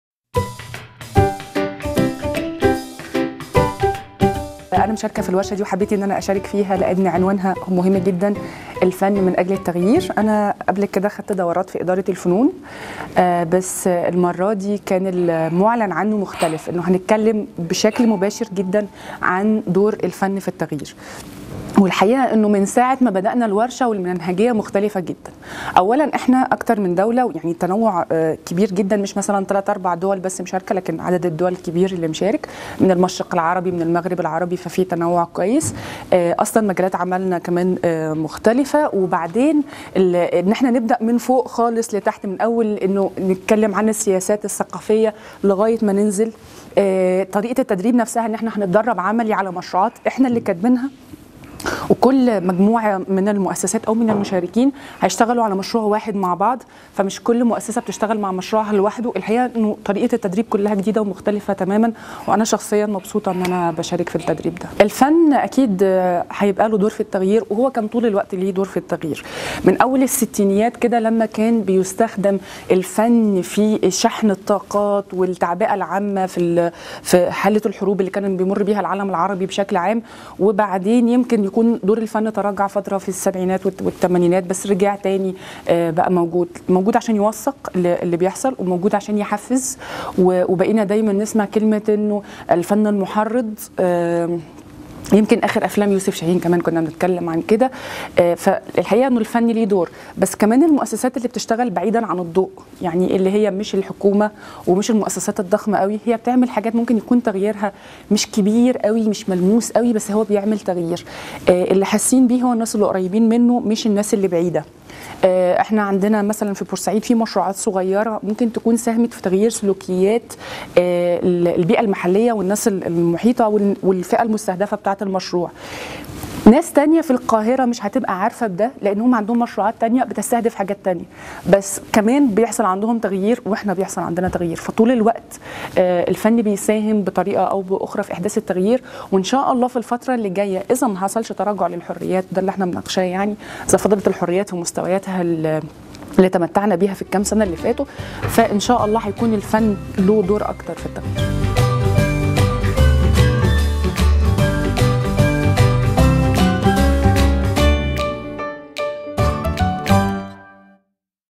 ‫#‏دورك: بدر العبيدي، مؤسسة مبادرون للثقافة والإعلام
أثناء تدريب "الفن من أجل التغيير"
1-6 مارس 2014، عمان، الأردن